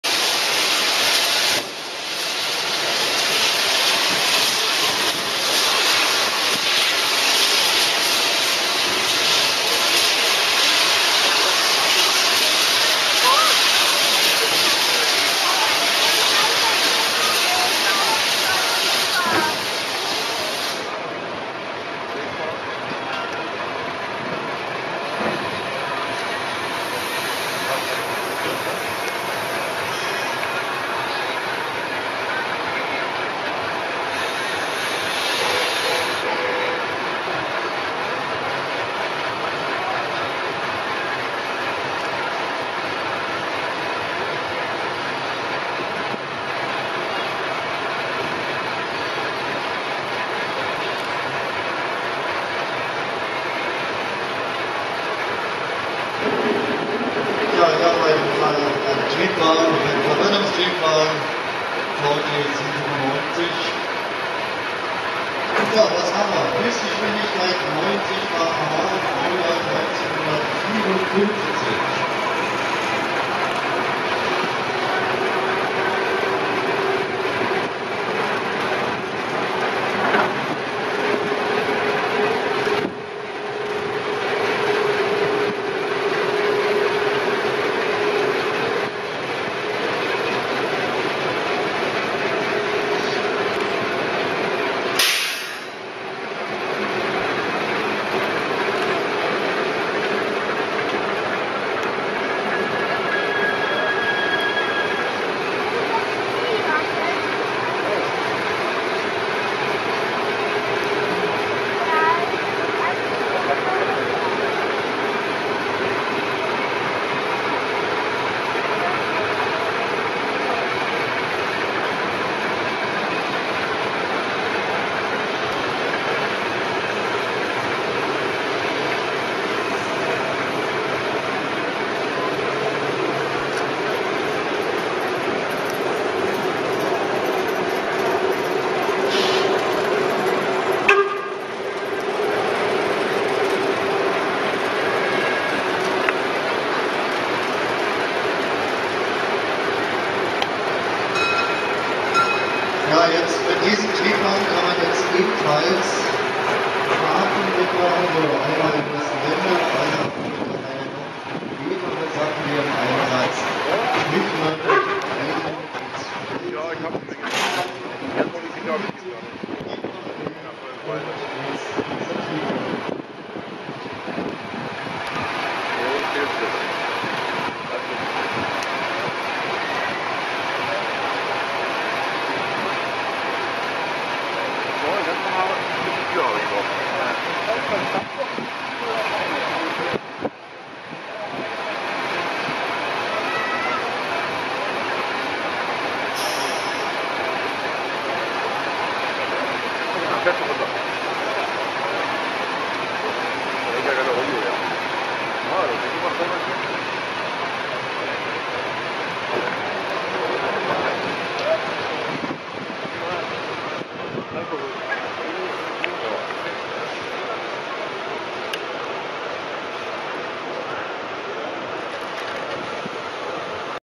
Schienenbus VT95 auf der Drehscheibe
Mein Besuch beim 6. Berliner Eisenbahnfest: Schienenbus auf der Drehscheibe